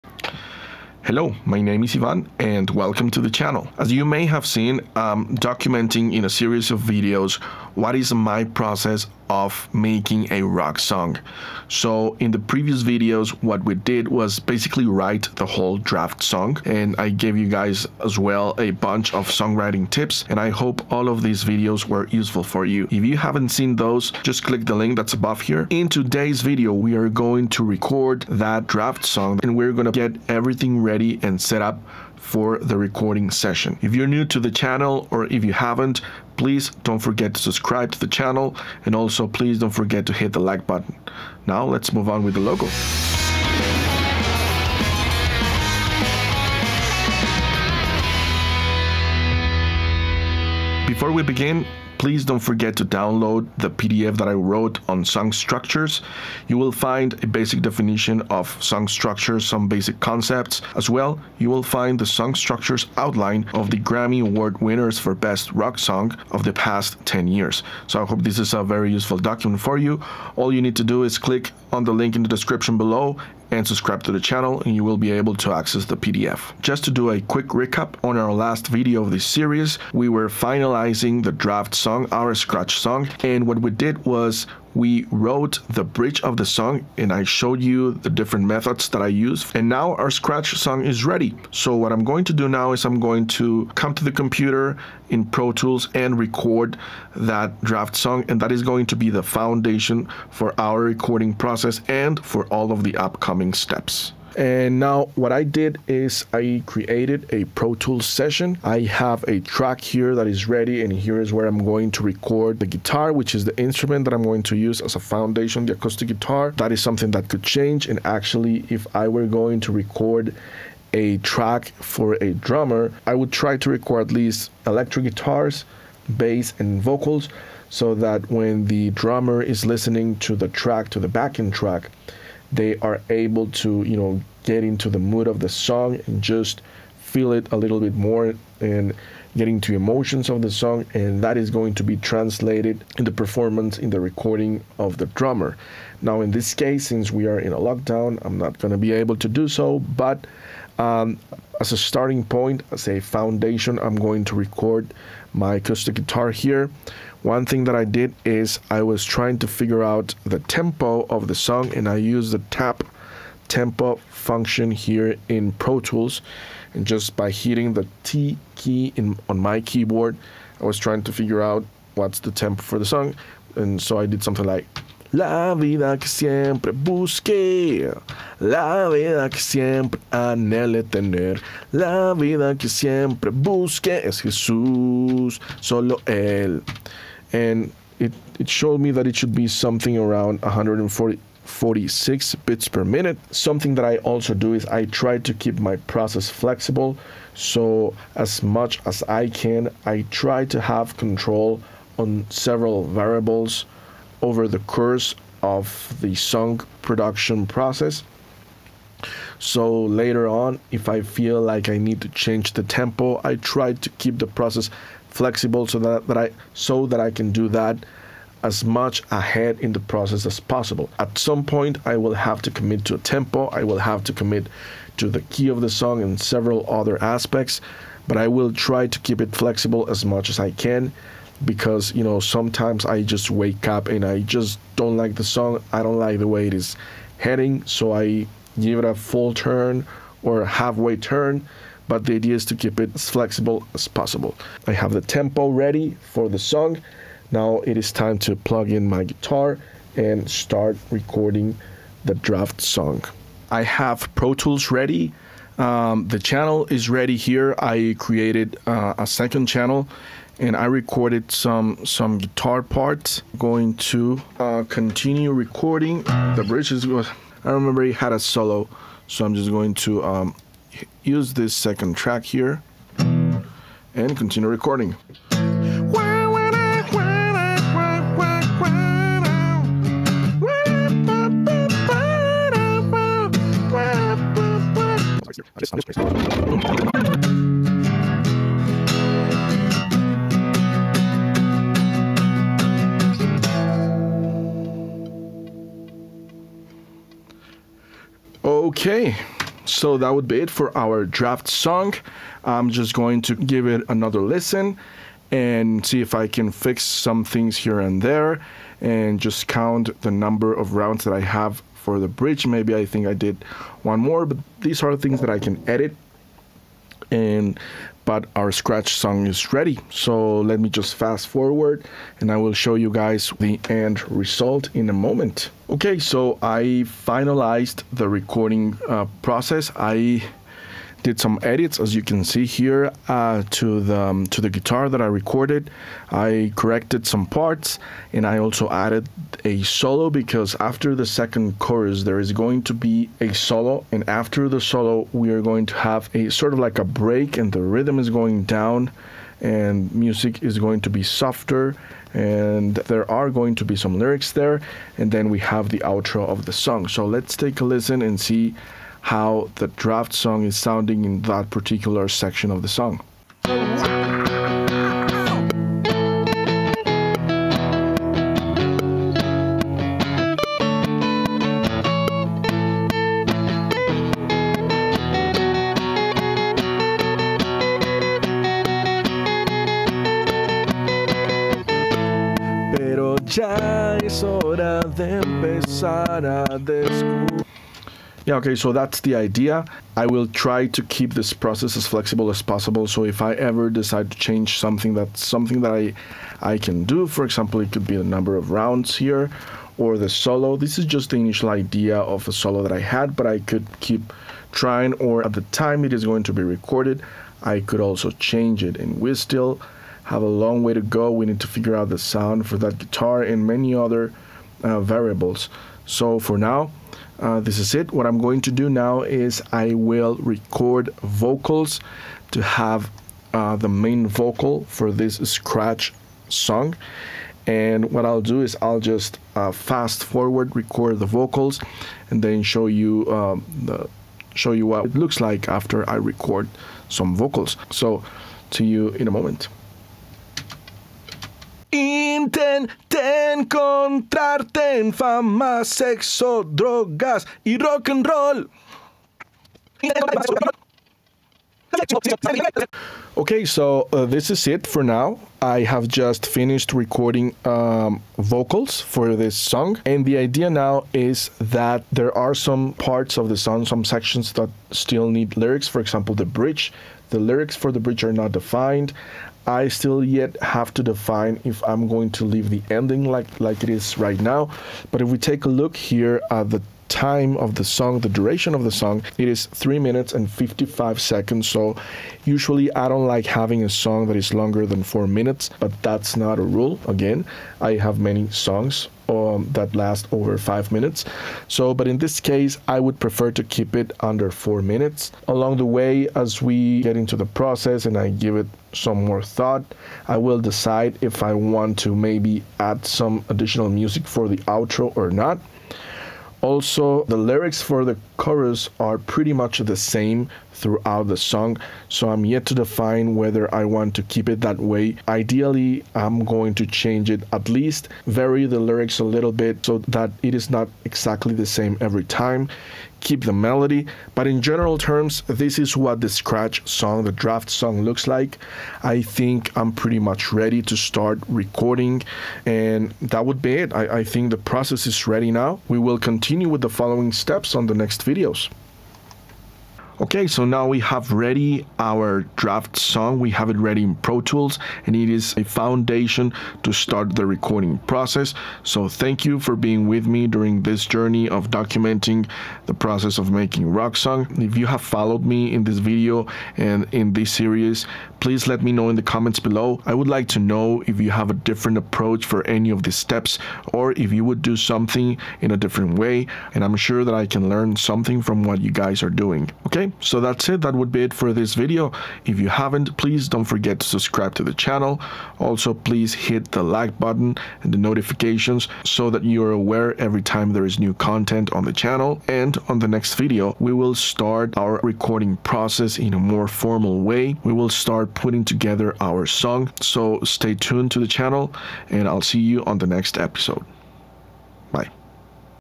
HOW TO RECORD A ROCK SONG - PART 1: Setting up the session in Pro Tools with a backing track.
Would you like to see the whole process behind making a rock song? Today I’m sharing with you my whole process. Starting from an idea I had recorded in a voice memo in my cell phone, all the way until the song is recorded, mixed, mastered and up in the digital platforms. This is the fourth video of my series. In the previous videos we wrote the draft song, now in this video I’m going to record it into Pro Tools and set it up as a guide for recording other instruments.

Sound Oceanica: Is a music production company. In this channel we share tips and document everything related to this topic.